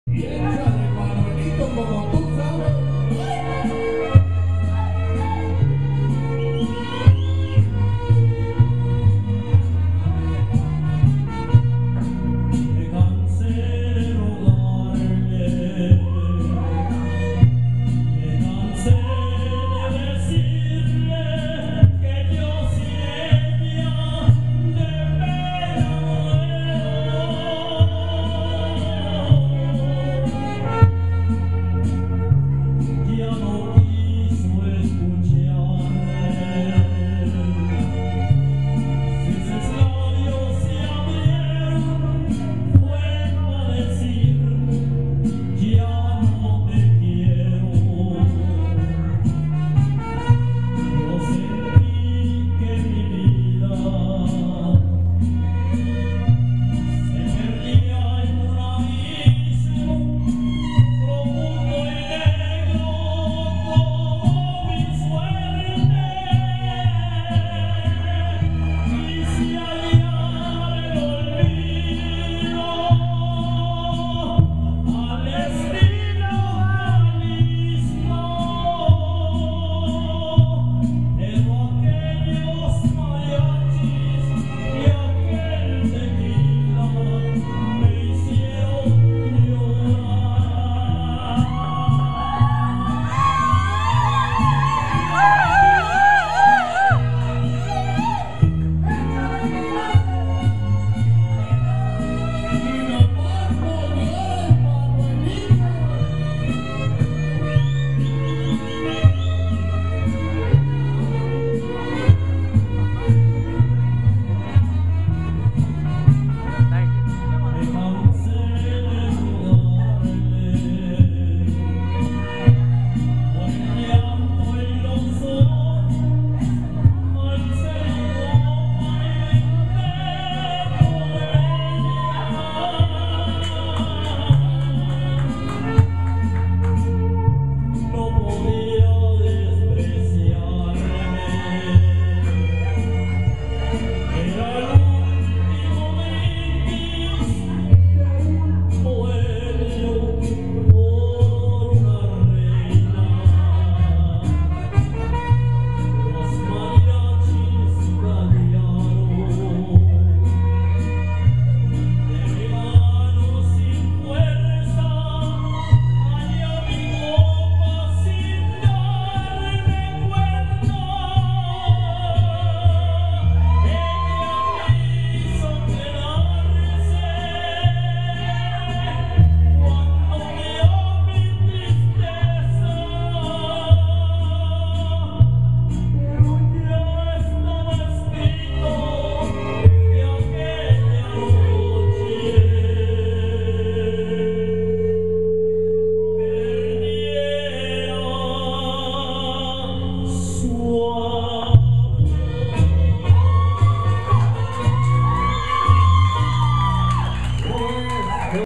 Vicente Fernandez 2011
Manuel singing karaoke
HONOR A VICENTE